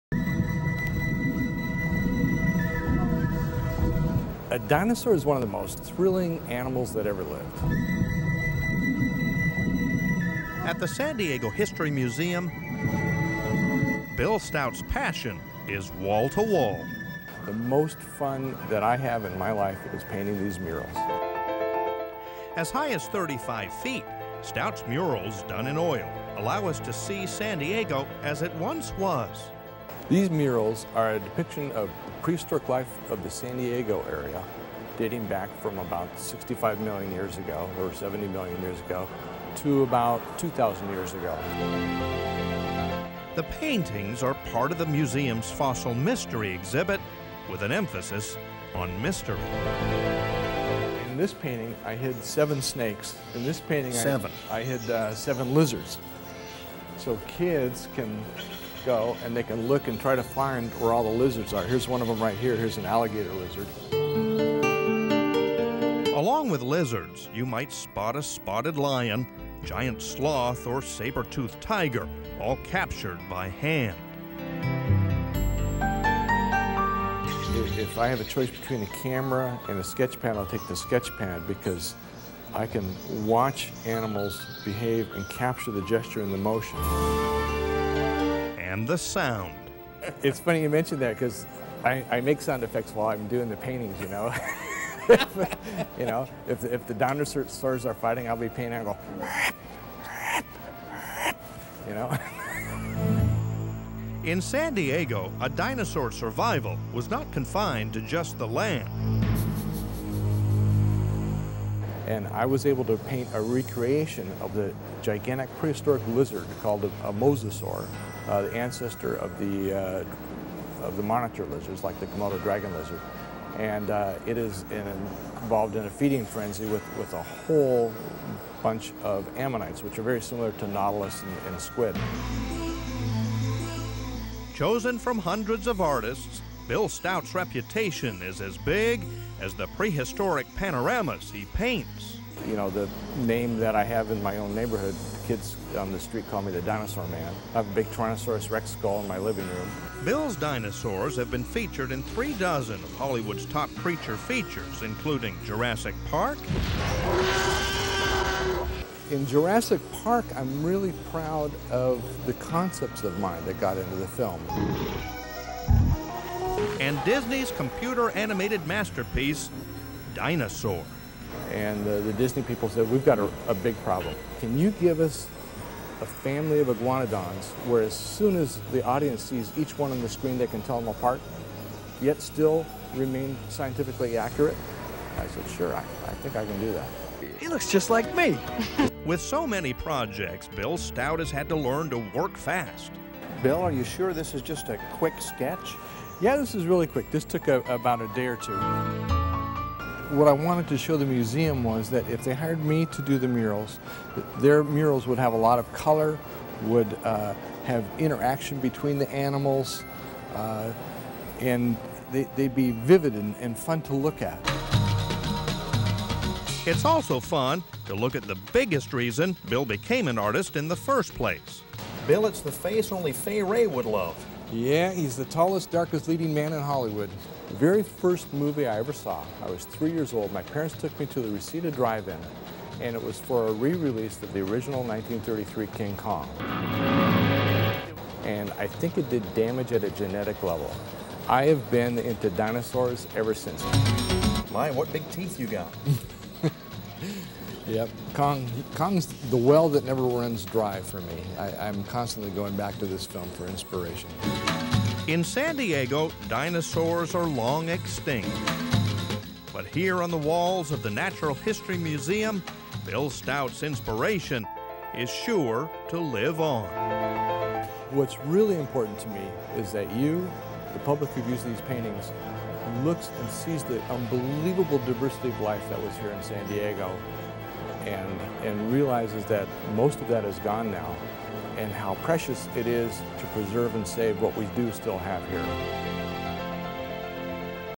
Bill Stout Graphic Artist
William Stout designed and produced the large graphic images of prehistoric scenes in the San Diego region for the San Diego Natural History Museum